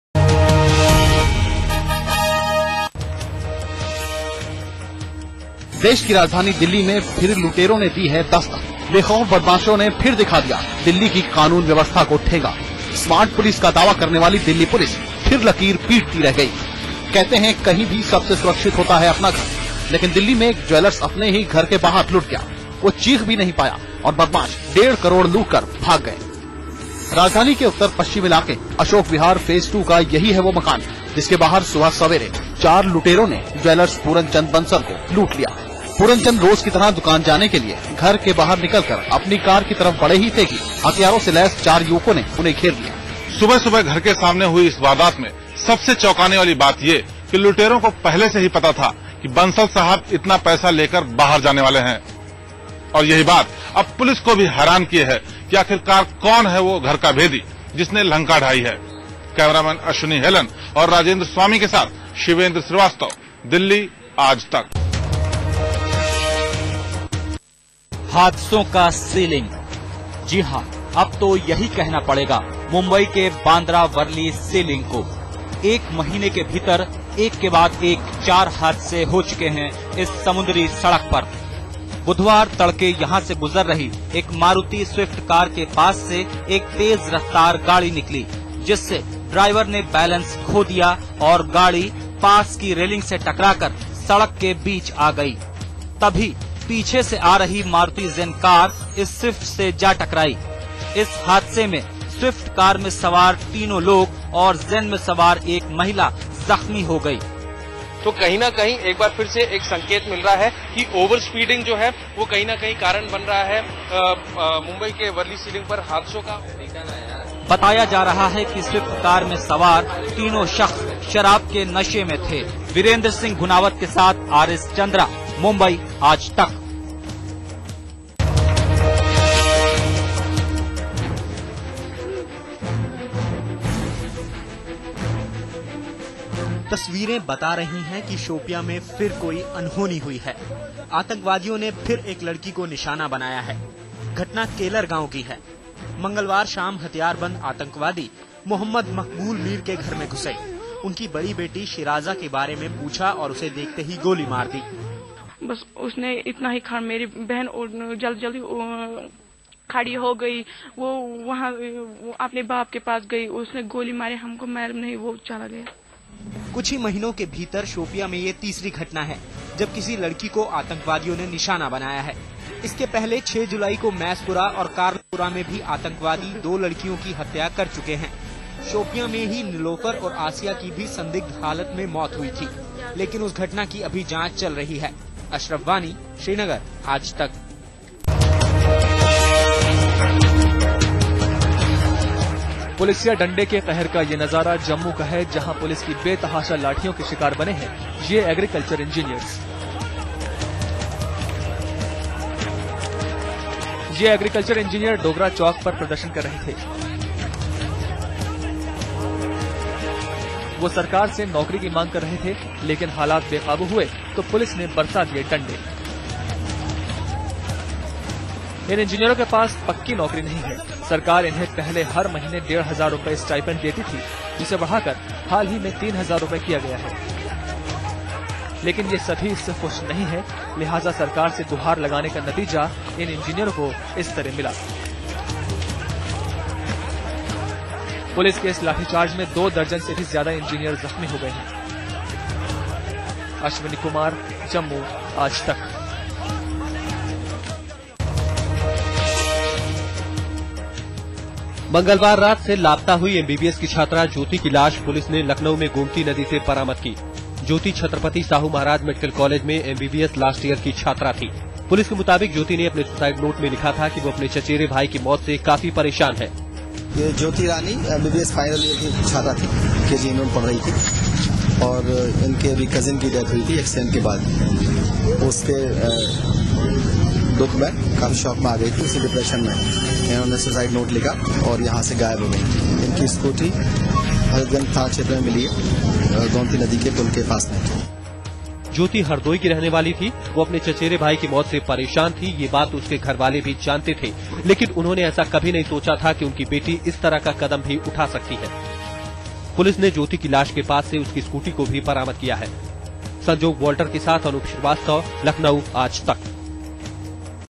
Trader robbed of Rs 1.5 crore in Delhi
Four assailants robbed trader Puranchand Bansal of Delhis Ashok Vihar of Rs 1.5 crore when he left his house to go to his shop. The four, who were on motorbikes, robbed him when he was about to reach his car.